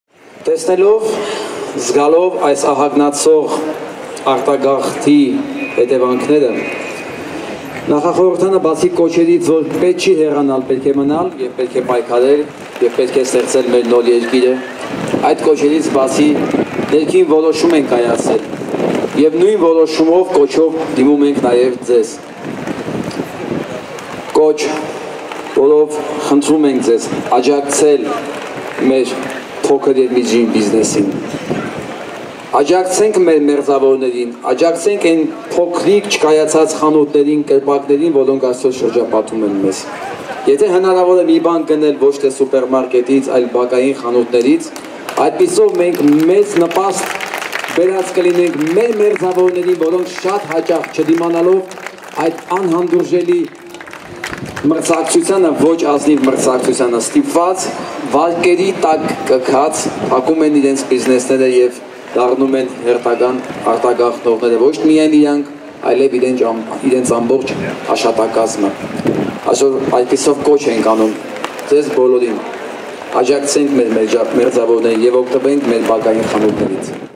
Նախախորհրդարանի կոչը փոքր և միջին խանութներից գնումներ կատարելու վերաբերյալ
Օլիգարխներին պատկանող սուպերմարկետները, օգտվելով իրենց մենաշնորհային հնարավորություններից, անհավասար մրցակցության միջոցով գիշատչաբար դուրս են մղում փոքր և միջին խանութներին` դրանց սեփականատերերին ստիպելով փակել իրենց բիզնեսը: Նրանցից շատերը սնանկանում են և բռնում արտագաղթի ճանապարհը:
Նախախորհրդարանը, մտահոգված լինելով այս իրավիճակով, 2013 թ. հուլիսի 12-ին  Ազատության հրապարակում կազմակերպած հանրահավաքի ժամանակ կոչ է արել ընտրության հնարավորության դեպքում սուպերմարկետների փոխարեն գնումներ կատարել փոքր և միջին խանութներից, ինչի շնորհիվ մենք կարող ենք օգնել փոքր կամ միջին բիզնես ունեցող և ժողովրդի մեծամասնության դժվարին կյանքով ապրող մեր մերձավորին ու նրա ընտանիքին, նպաստել արտագաղթի նվազեցմանը, ինչպես նաև չհարստացնել մեզ կեղեքող օլիգարխներին: